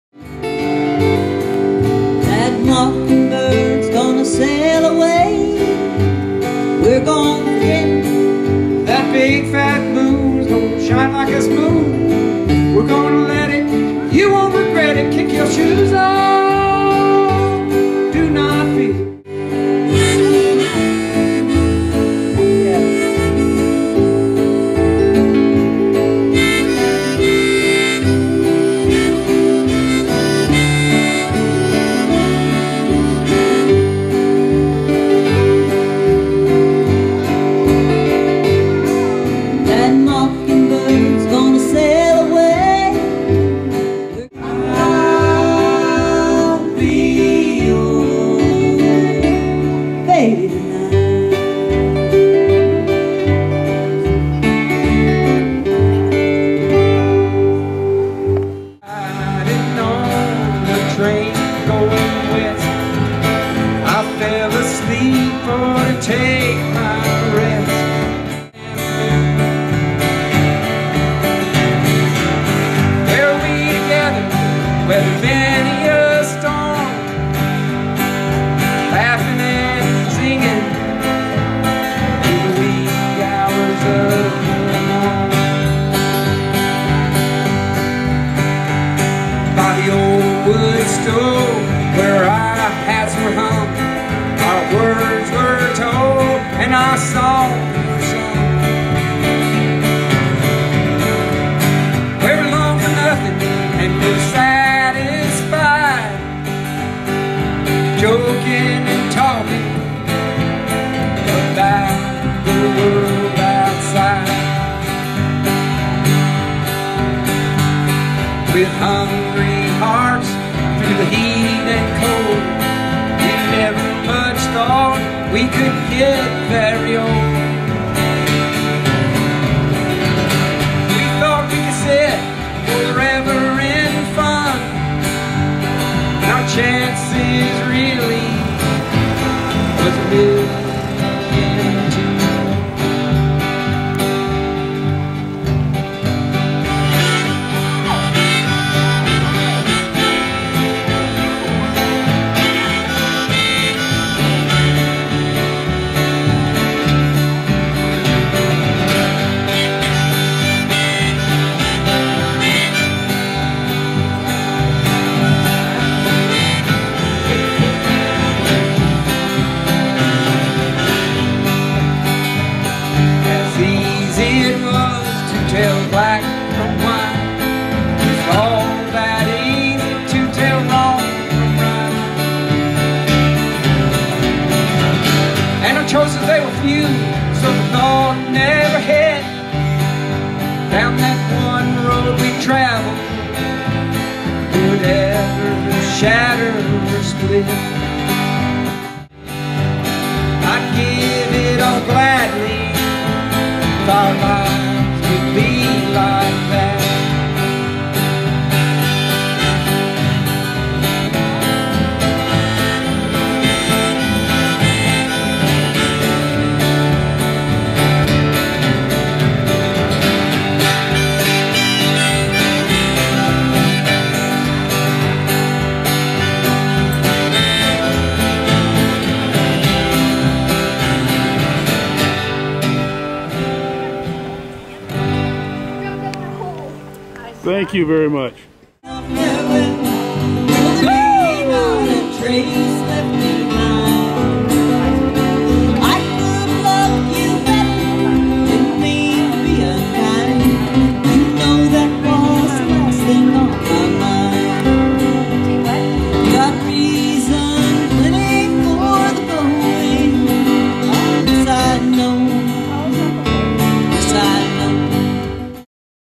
Lucky Streak
Lucky Streak performs at Mountain Air Family on the Green